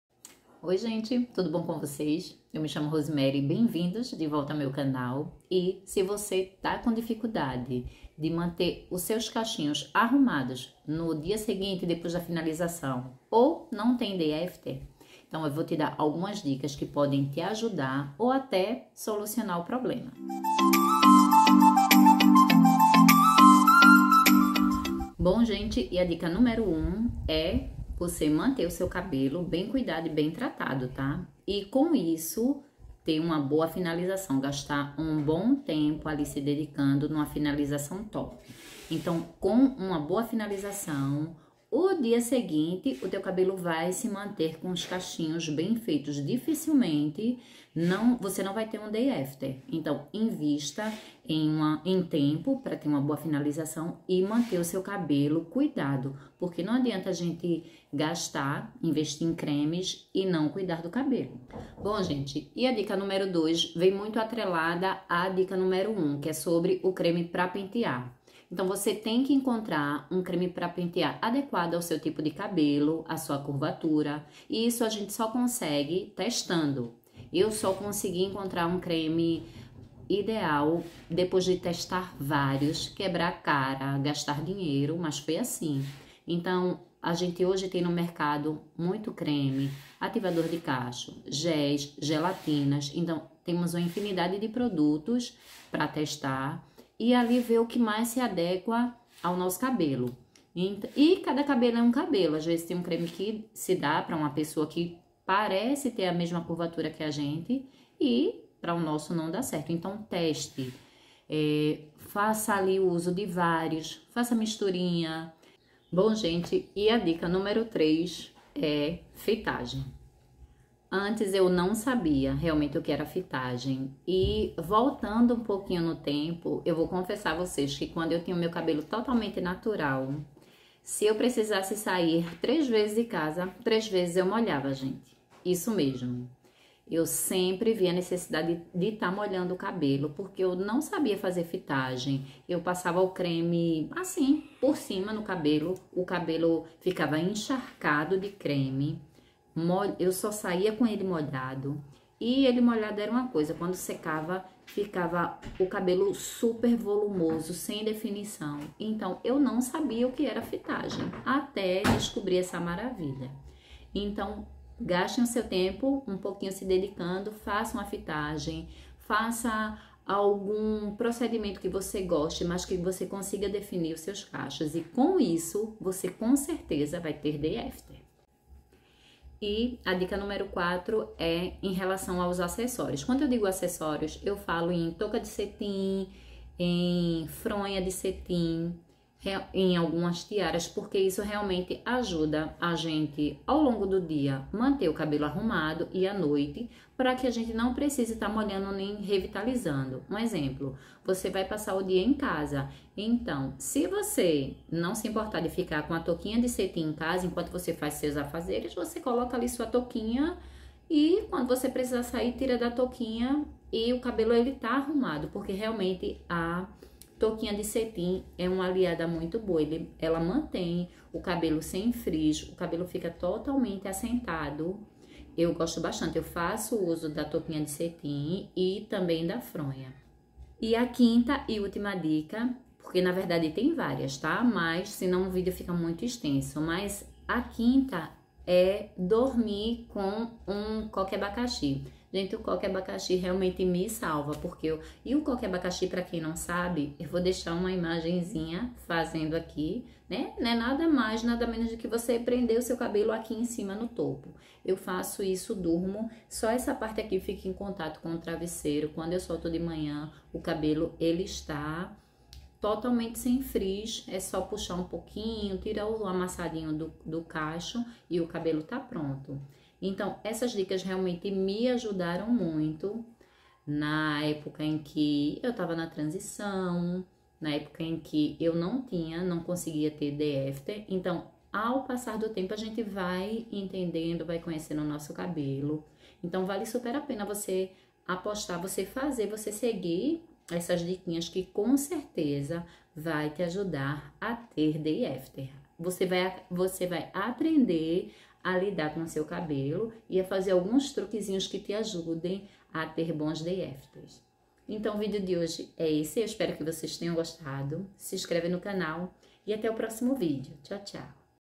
NÃO CONSEGUE TER DAY AFTER?VEM QUE EU TE AJUDO!!!cabelocacheado cabeloscrespos day after
Oi gente, acho que o sonho de toda cacheado é crespa é acordar com o cabelo perfeito ,sem friz e pronto para qualquer evento.
Quem nunca se atrasou porque o cabelo não estava nos seus dias bons.
Já passei por tudo isso e hoje trago algumas dicas básicas para nós crespas e  cacheados  conseguirmos acordar com o cabelo lindo ou pelo menos mais fácil para modelar sem a necessidade de lavar novamente.